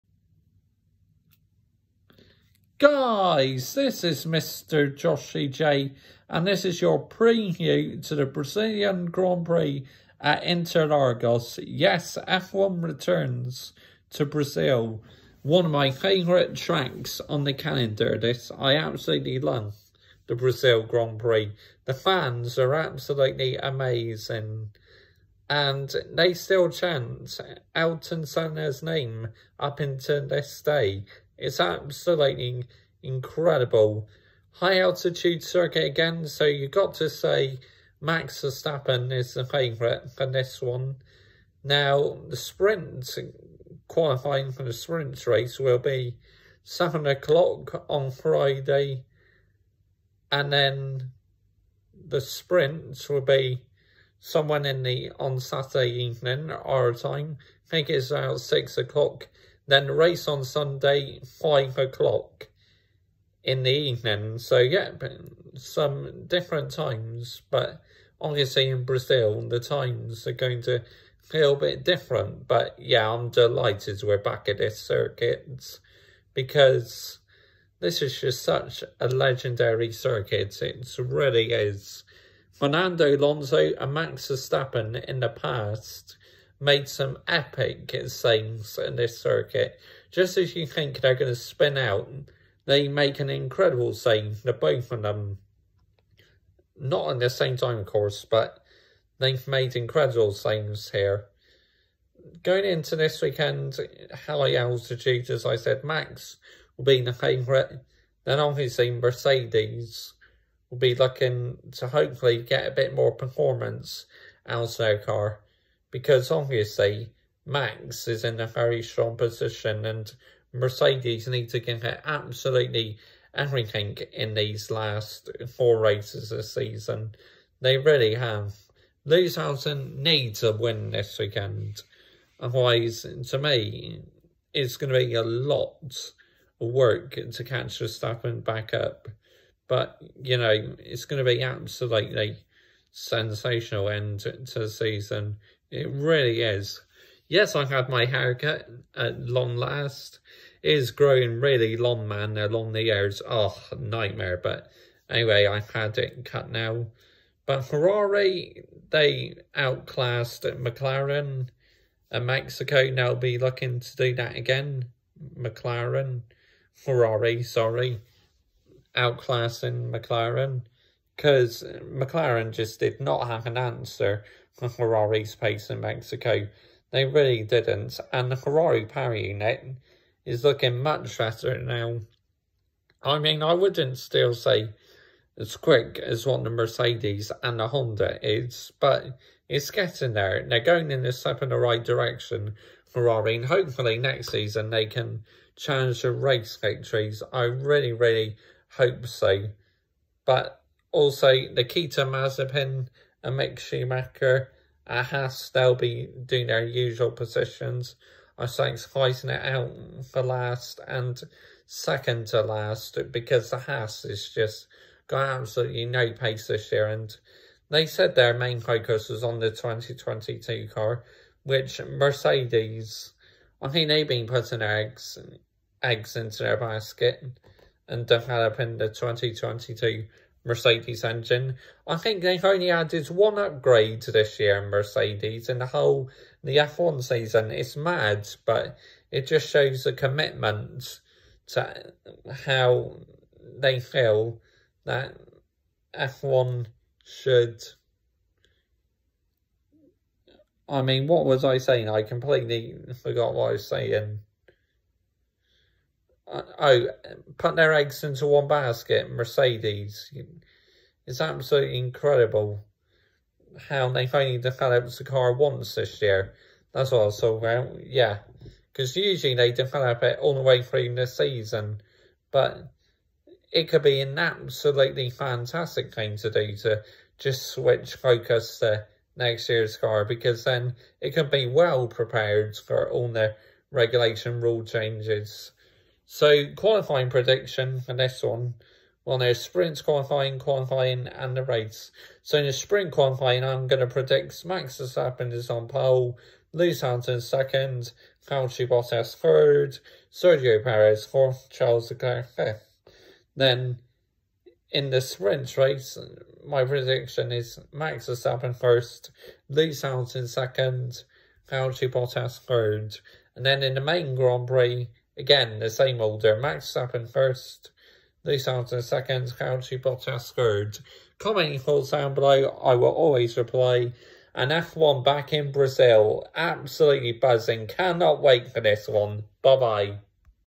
F1 2021 Brazil Grand Prix Preview Absolutely Love This Track So Excited
F1 2021 Season arrives in Brazil this Track is absolutely amazing so happy to have it back this Season really missed it last Year Red Bull will be strong favourites again this Weekend another high altitude Track this is a massive Weekend for Lewis Hamilton to keep his hopes alive. My Sprint Qualifying prediction is Max Verstappen 1st Lewis Hamilton 2nd Valtteri Bottas 3rd Perez 4th Leclerc 5th.